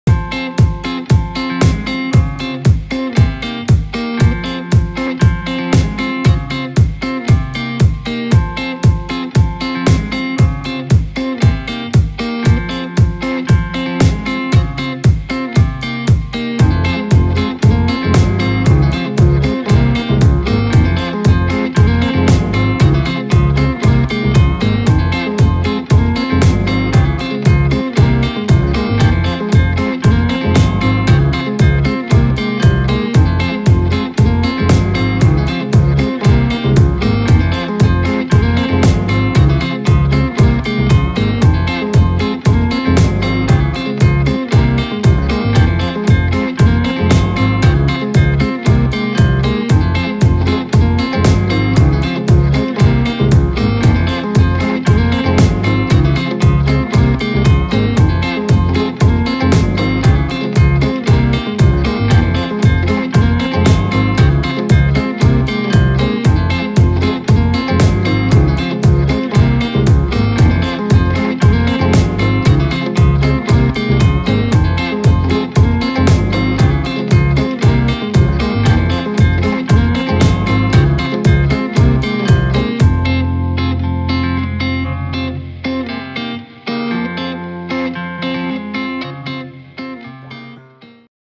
Karthalle Marktl Sonntag 10.11.2019 RYR 🏁 Freiwillige Feuerwehr Stein a.d. Traun Kartrennen
🏆RYR - „remember your race“ Video vom Sonntag 10. November 🍀🏁

@FFW Stein a. d. Traun 🏁 schnell ...schneller 🚒 beim Formel 3 Kart RACE 🏁